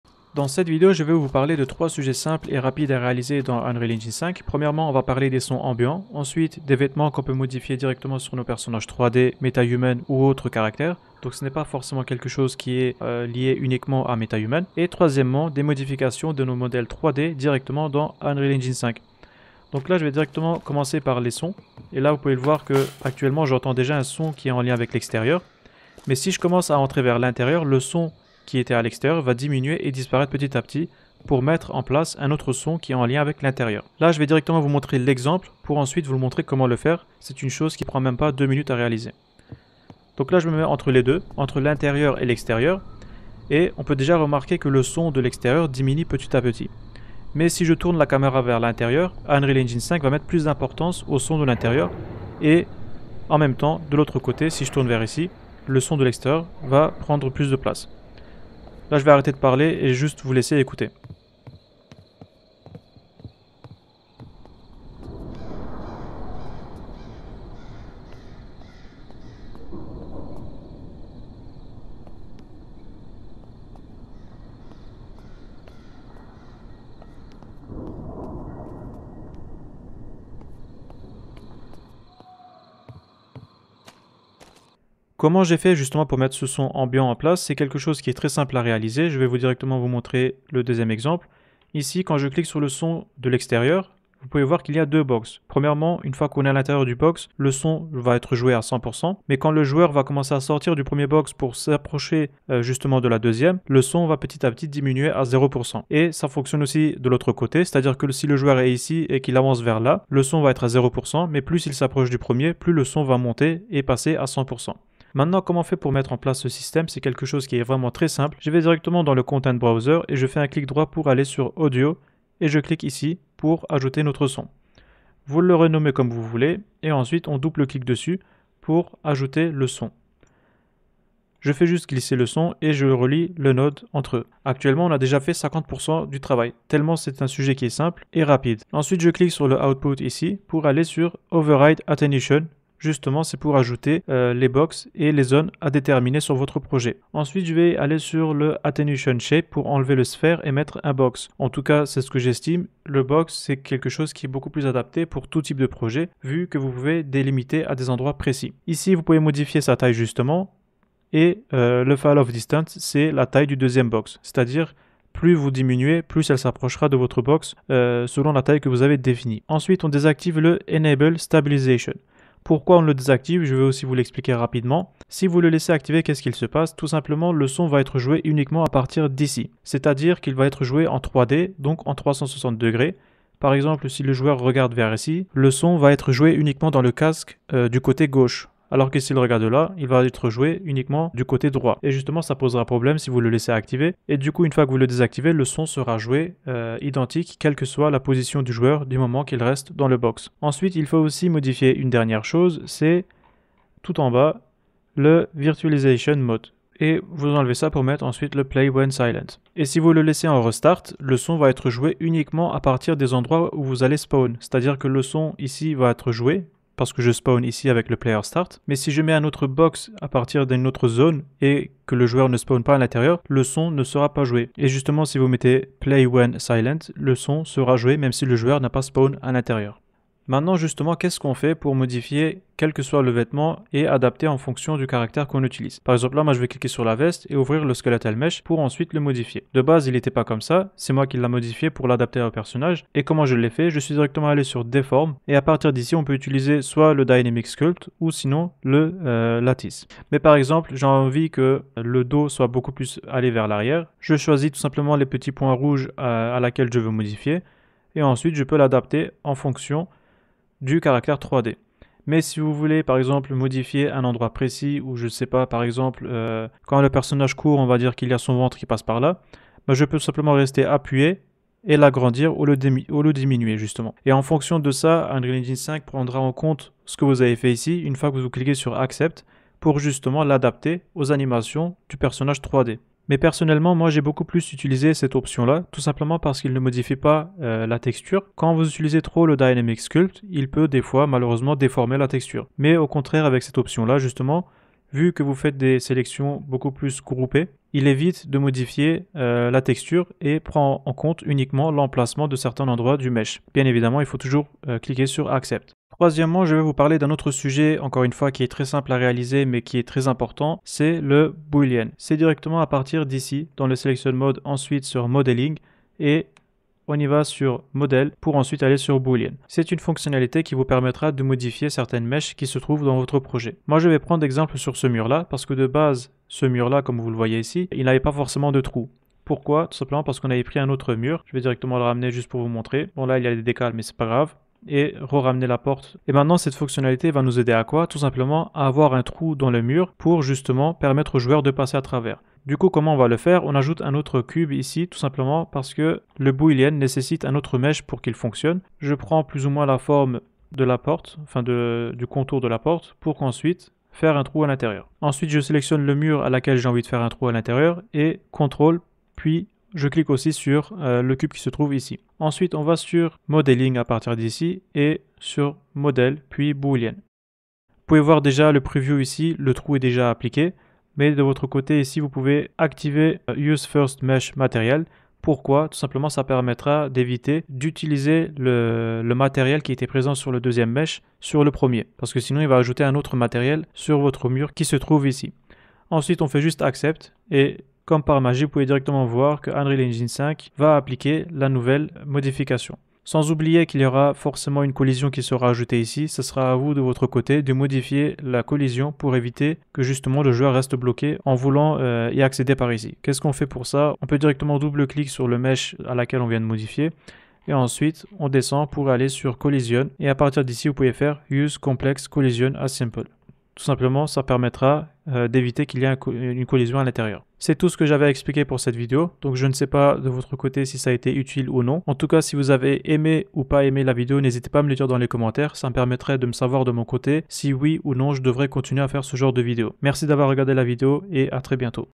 3 Astuces Faciles pour Vos Projets Unreal Engine 5 : Son, Vêtements & Modélisation 3D
Dans cette vidéo, je vous montre 3 astuces simples et rapides pour améliorer vos projets Unreal Engine 5. Vous apprendrez à :

00:00 Optimiser le son ambiant pour une immersion totale.
03:51 Modifier facilement les vêtements des personnages.
05:07 Retoucher directement vos modèles 3D dans Unreal.

Ces techniques sont faciles à appliquer et feront une vraie différence dans vos projets.